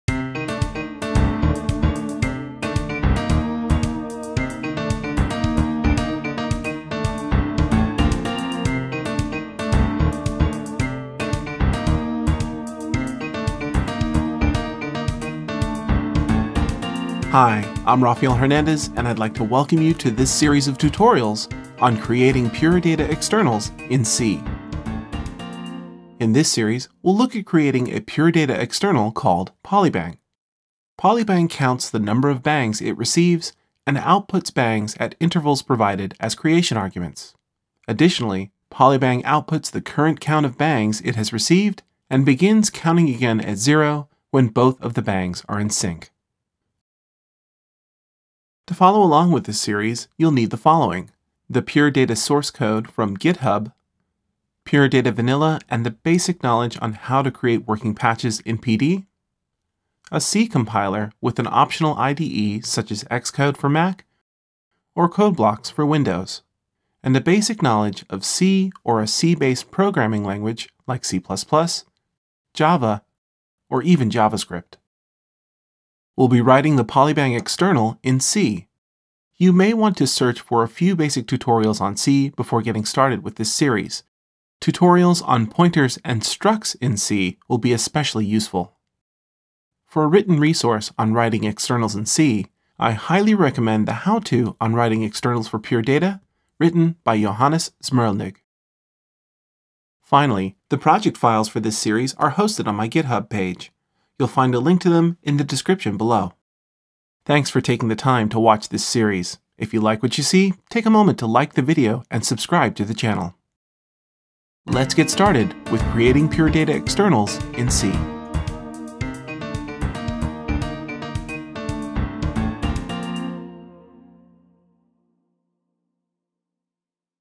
Creating Pure Data Externals in C (2016): 00. Introduction
Introduction to a series of tutorials on creating a simple control-rate external for Pure Data.